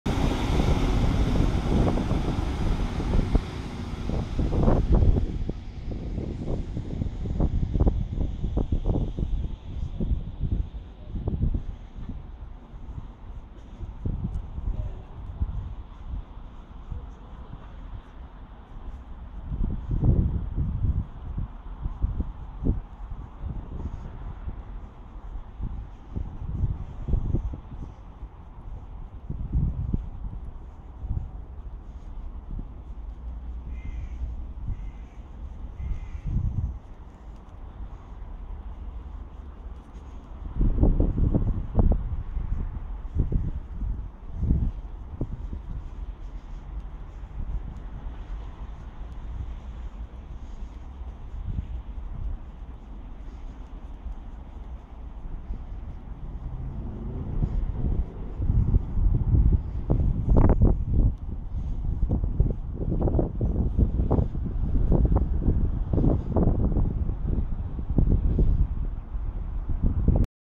Chorley Train Station
Train Service From Blackpool North To Manchester Airport Calling At Chorley